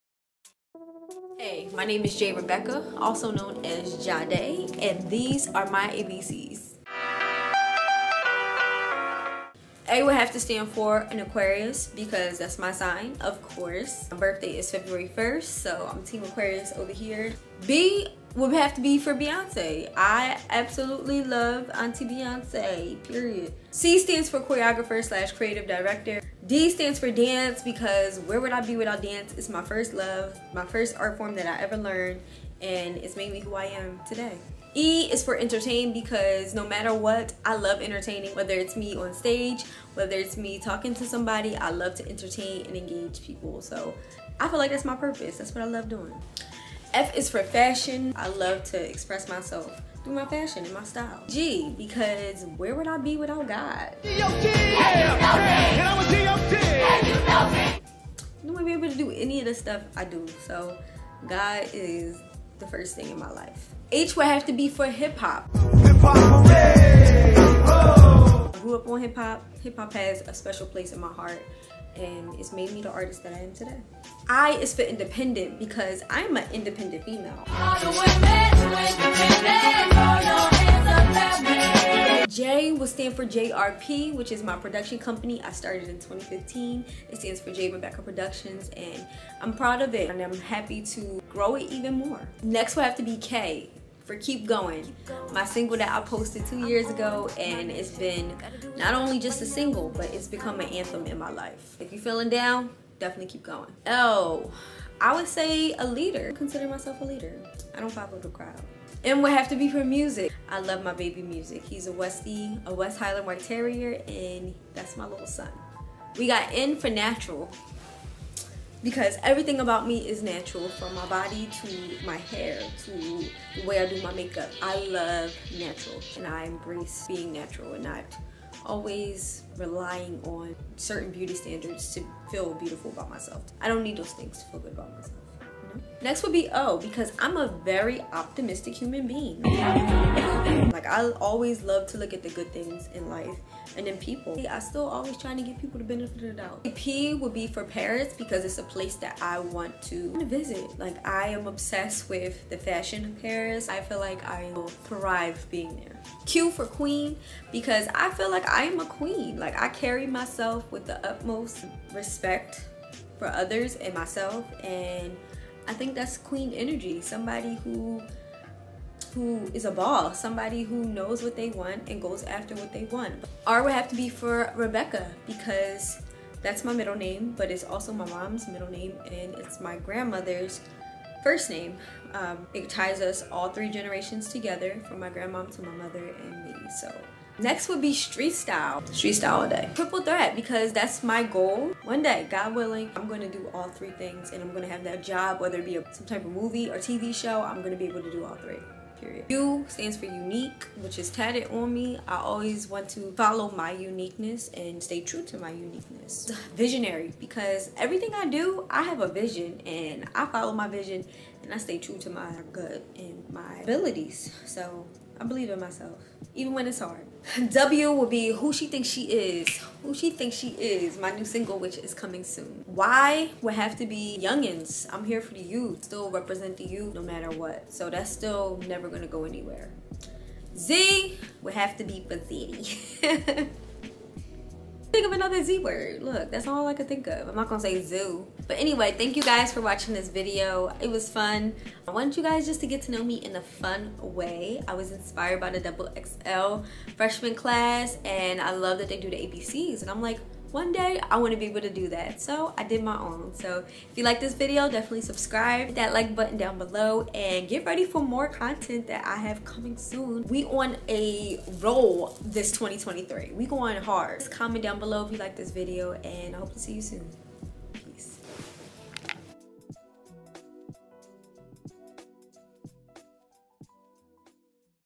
Jade's ABC'S - GET TO KNOW ME TAG
If I was on the xxl fresh man class these would be my ABC'S.  Get to Know me a little better.